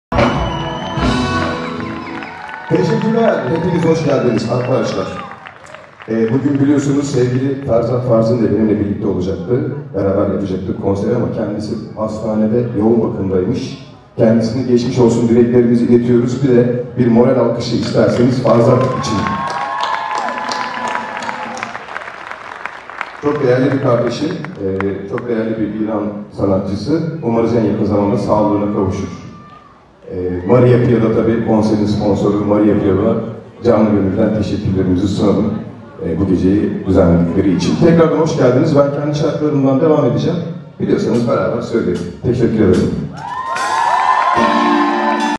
ابراز تأسف «الیاس یالچینتاش» از عدم حضور «فرزاد فرزین» بخاطر بیماری در کنسرت امشب، ۱۱ فروردین
🔹 ابراز تأسف «الیاس_یالچینتاش» از عدم حضور «فرزاد فرزین» بخاطر بیماری در کنسرت امشب، ۱۱ فروردین در استانبول و آرزوی سلامتی برای فرزاد.
ساعت های سختی را فرزاد در حال گذراندن است و امیدواریم در ساعات آینده اخبار خوبی از اوضاع جسمی این خواننده منتشر کنیم. به نظر می‌رسد در ساعات اخیر اوضاع فرزاد نسبت به ساعات گذشته بهتر شده.

هر چند فرزاد و تیم اش، برای این کنسرت ماه ها تلاش کرده بود ولی قطعا با سلامتی کامل، باید اتفاقات بزرگتری از فرزاد را شاهد باشیم.
 زود خوب شو آقای آرتیست، زود بلند شو که خیلی کارها رو زمین مونده...